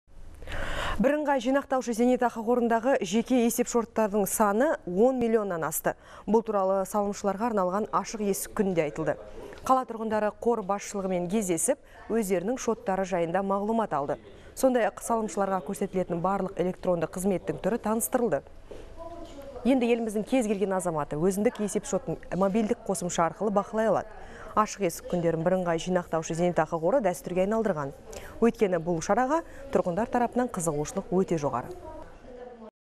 Зейнетақы қоры ашық есік күнін өткізді
Бірыңғай жинақтаушы зейнетақы қорындағы жеке есепшоттардың саны 10 миллионнан асты. Бұл туралы салымшыларға арналған ашық есік күнінде айтылды. Қала тұрғындары қор басшылығымен кездесіп, өздерінің шоттары жайында мағлұмат алды.